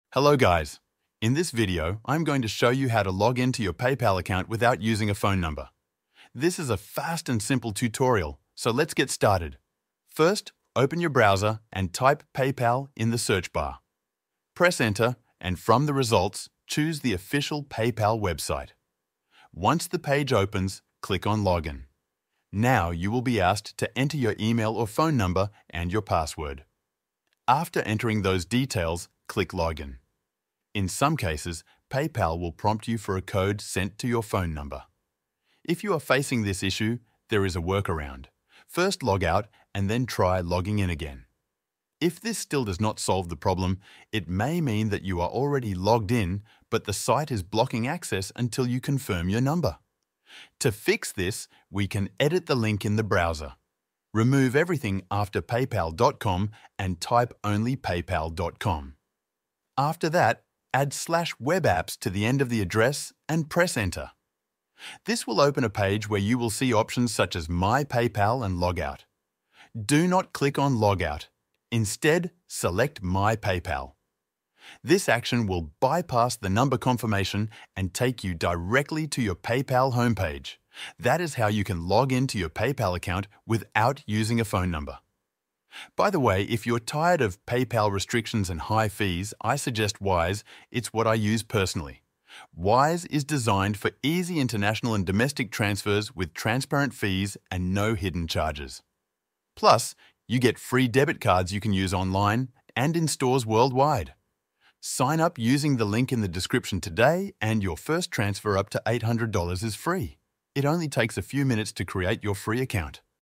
How to Login into PayPal Without Phone Number (2026) ✅
Want to know how to log in to PayPal without using a phone number in 2026? This step-by-step guide explains secure methods to access your PayPal account, manage your login options, and keep your account safe. Perfect for beginners or anyone looking for alternative ways to log in easily.